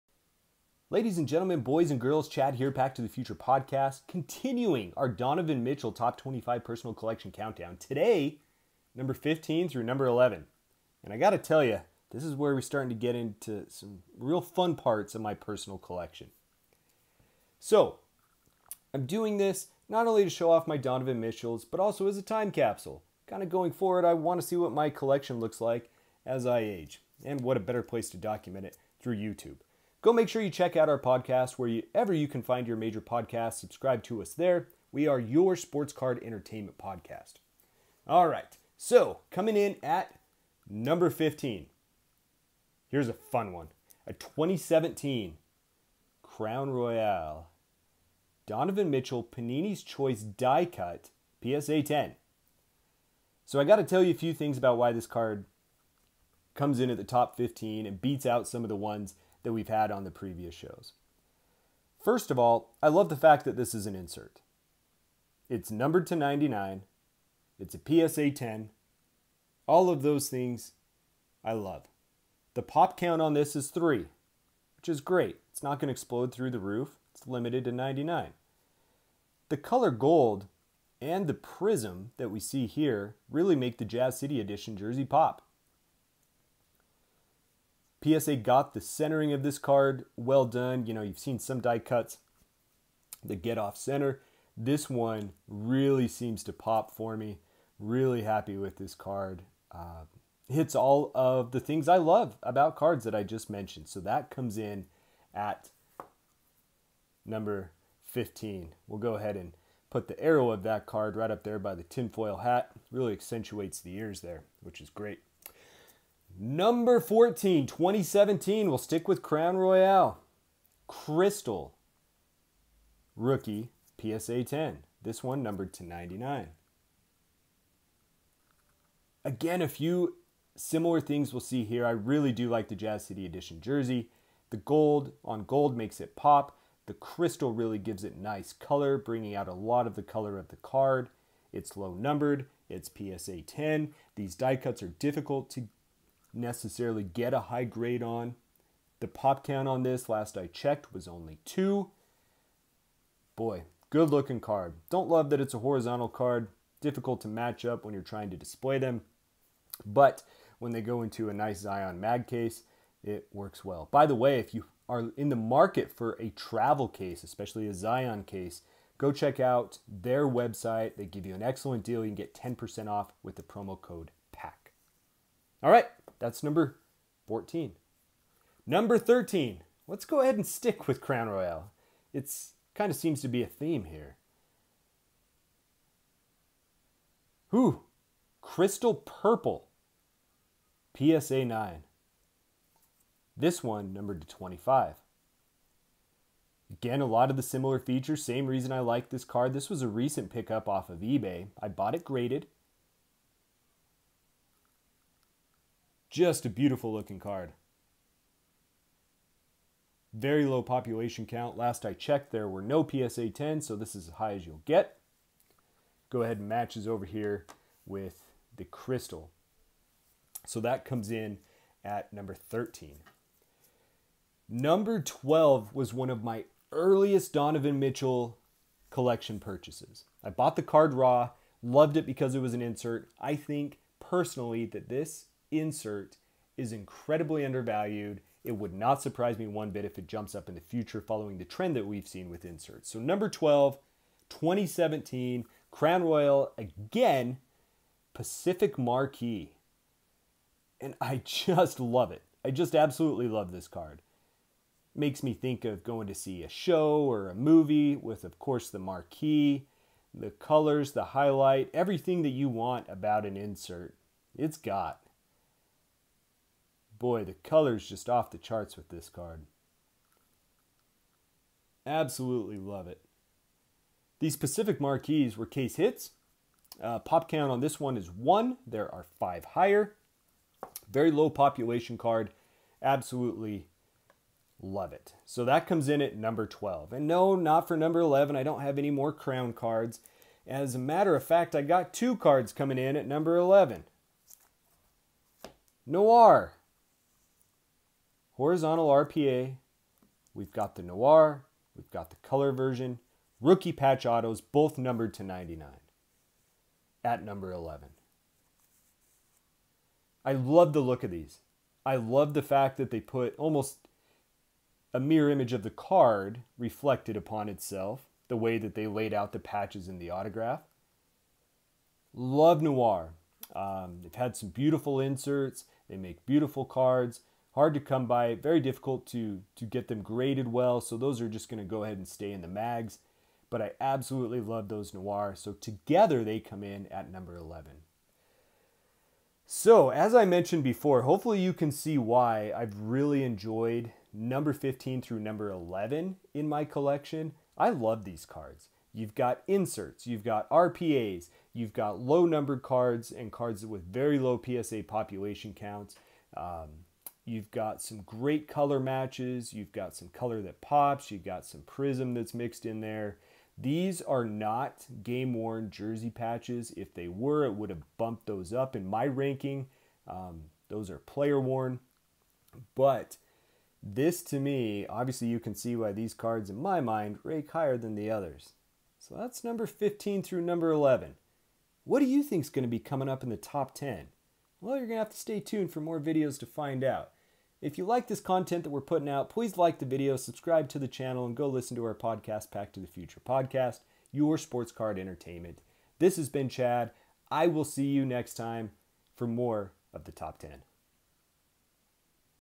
Donovan Mitchell Top 25 Personal Collection #15-11!
Chad from Pack to the Future Podcast ranks his Top 25 Donovan Mitchell PC!

YouTube: Pack to the Future Podcast